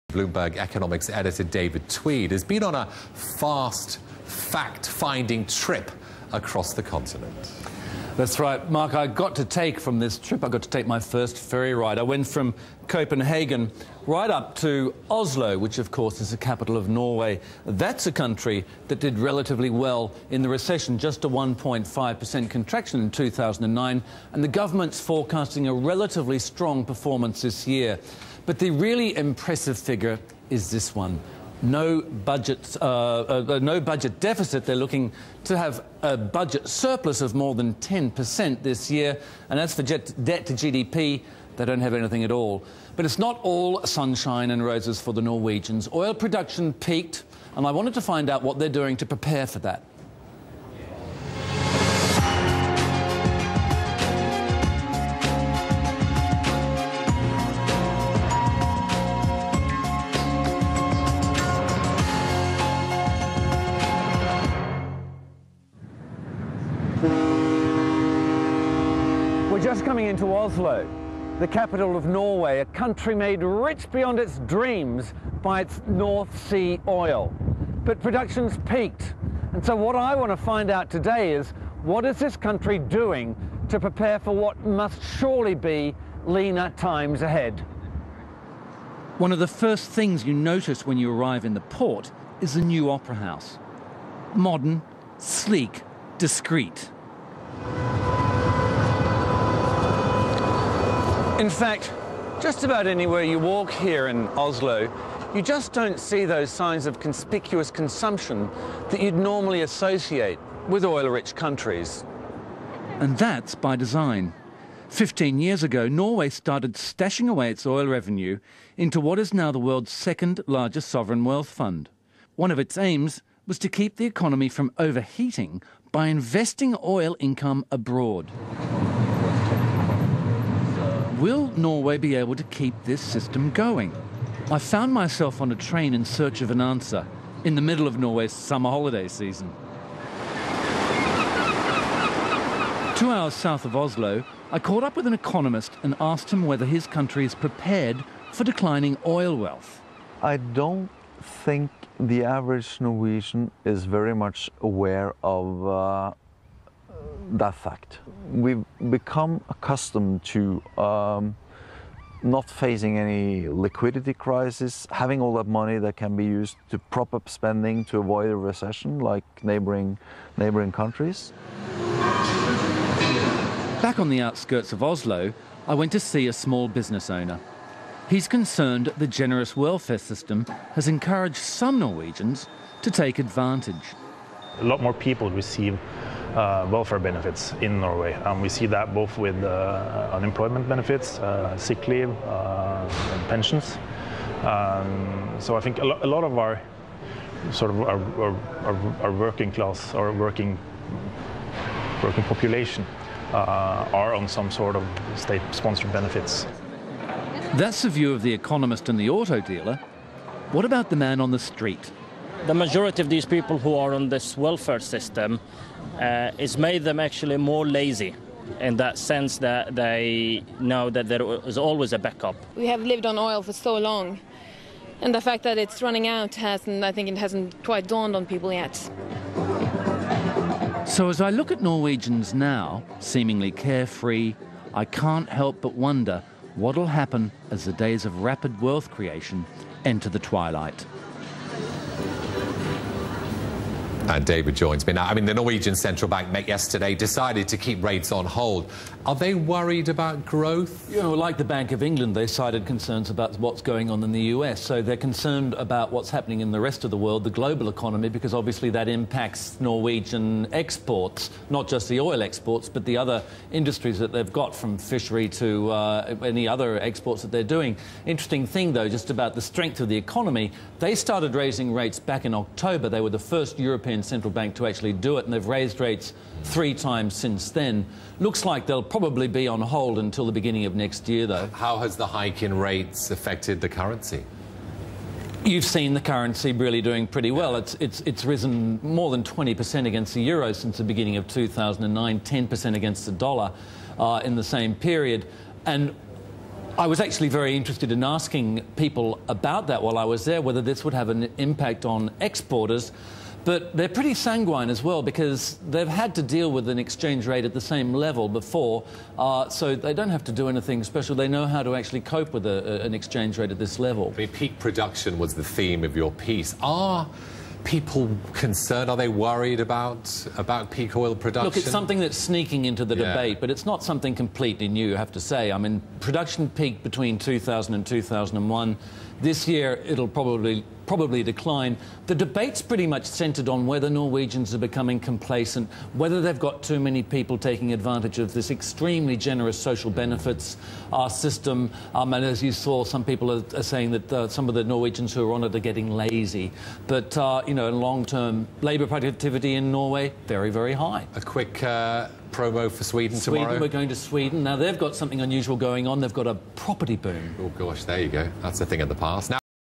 Norwegians Doubt Country Prepared for Life After Oil
Aug. 12 (Bloomberg) -- Bloomberg's David Tweed reports from Oslo on Norway's preparations for a decline in oil revenue.
     The Norwegian capital is Tweed's fourth stop on a train journey around Europe to survey the region's economy.